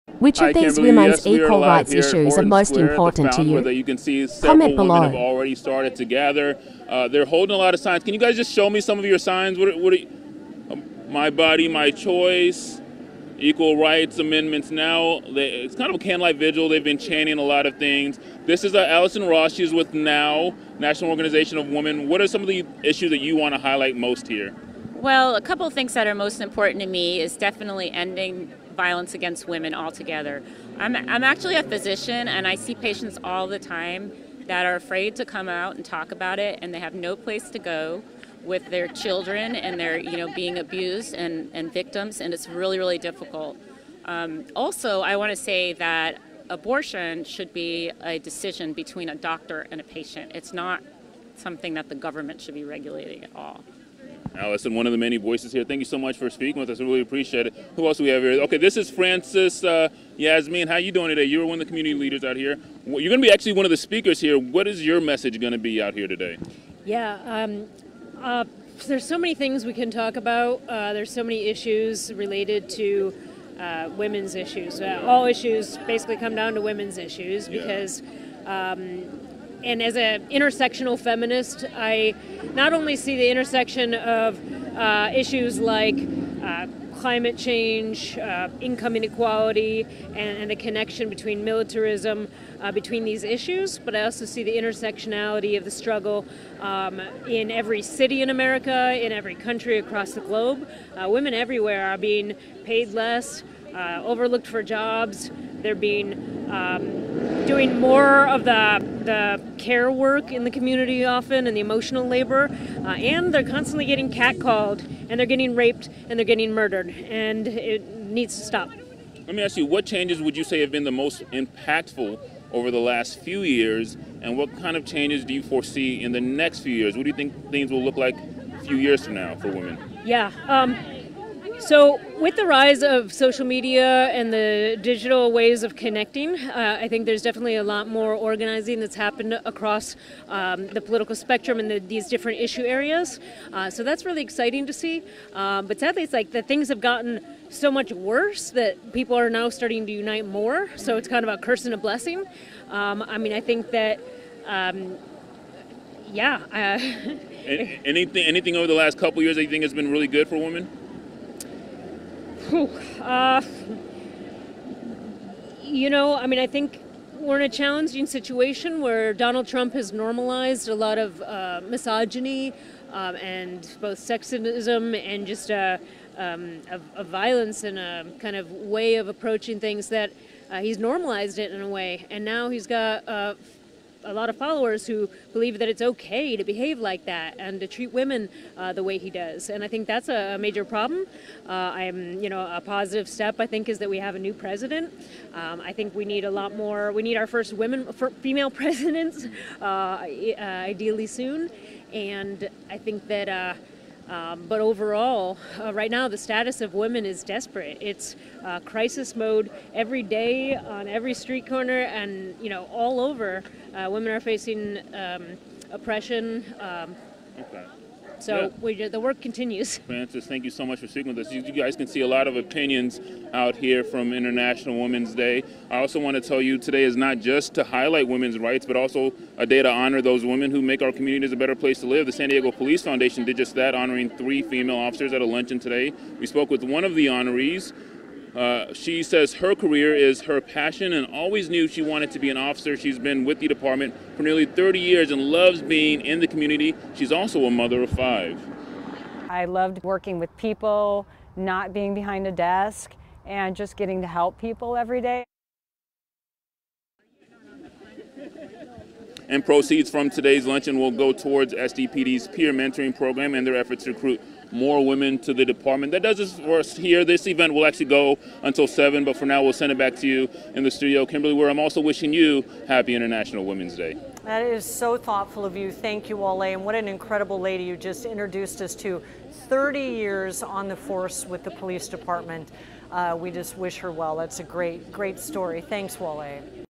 Women discuss most important gender equality issues for international women's day
equalPay abortion domesticviolence etc. Which of the many women's equal rights issues are most important to you?